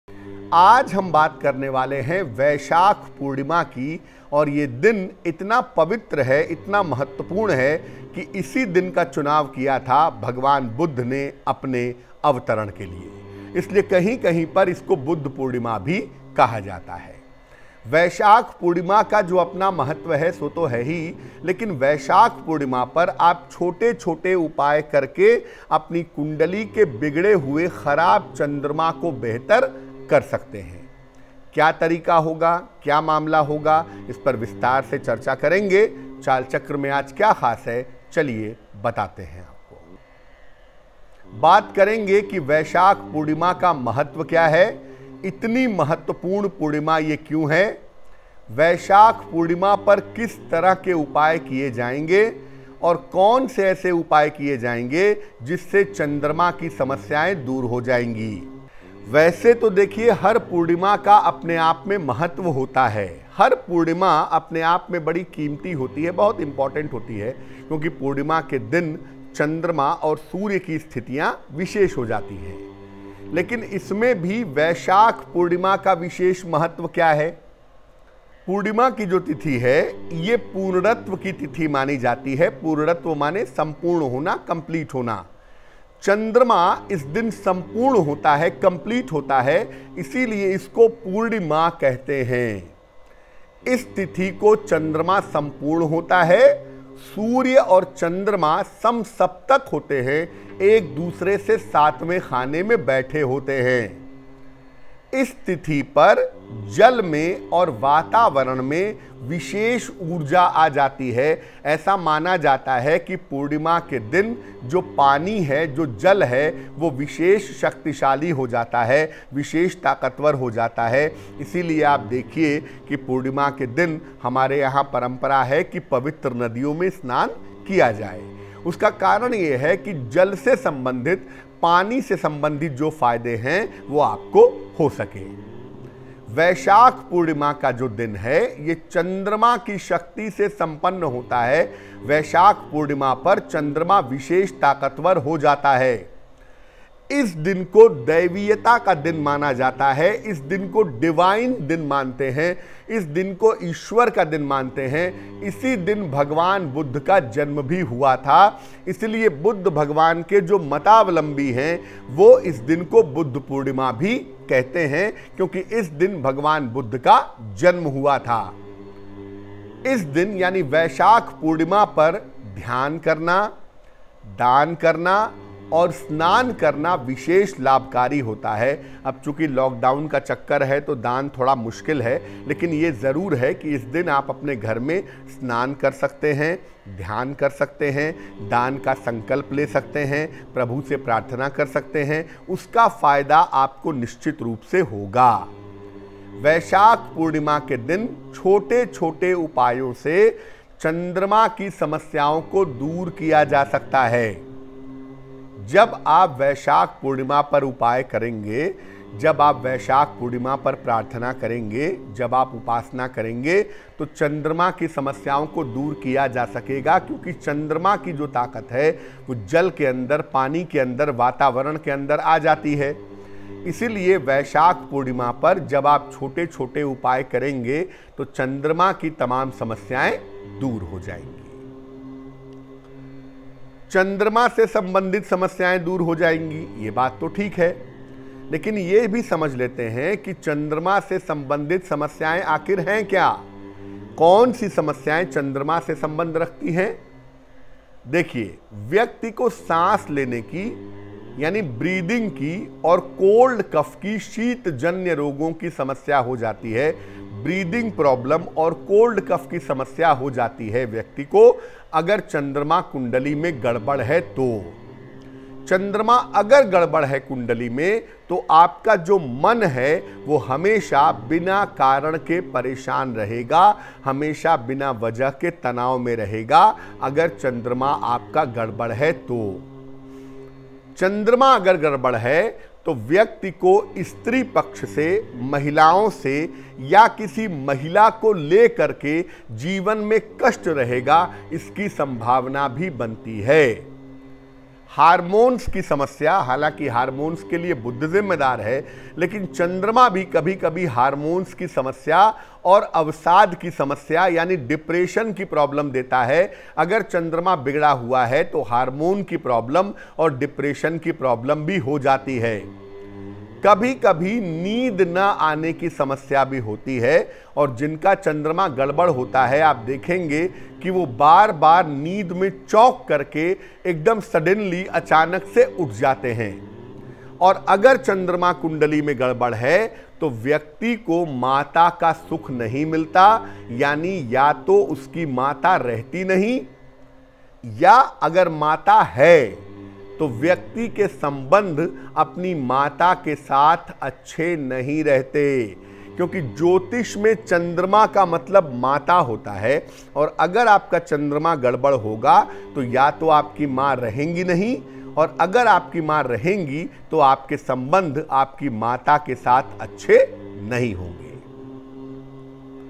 वैशाख पूर्णिमा की रात्रि करें ये प्रभावशाली उपाय | Vaishakha Purnima 2020 | Shailendra Pandey
ज्योतिर्विद शैलेंद्र पांडेय से जानिए वैशाख पूर्णिमा की रात्रि पर किए जानें वाले प्रभावशाली उपाय#DekhBinaBreak ShailendraPandey AstroTaK (Ask a Question)